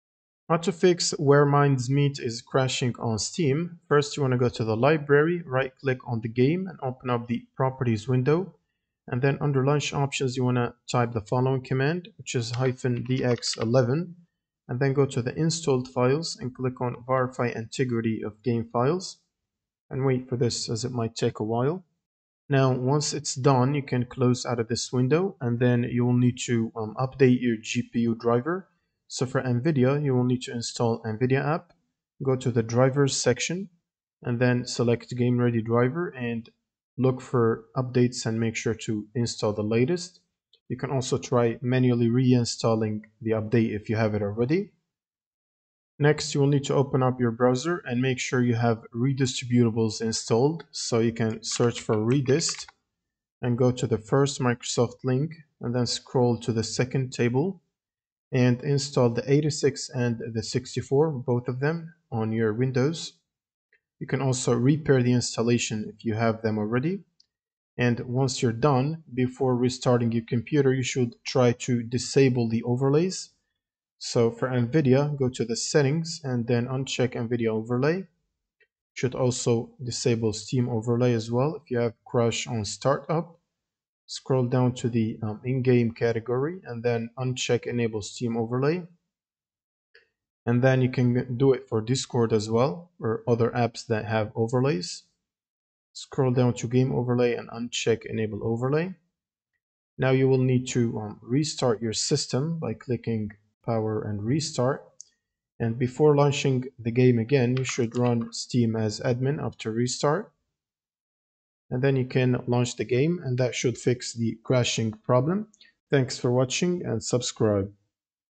How To Fix Where Winds Meet Crashing On Steam
Fix Where Winds Meet crashing on Steam with these proven troubleshooting steps for a stable gaming experience.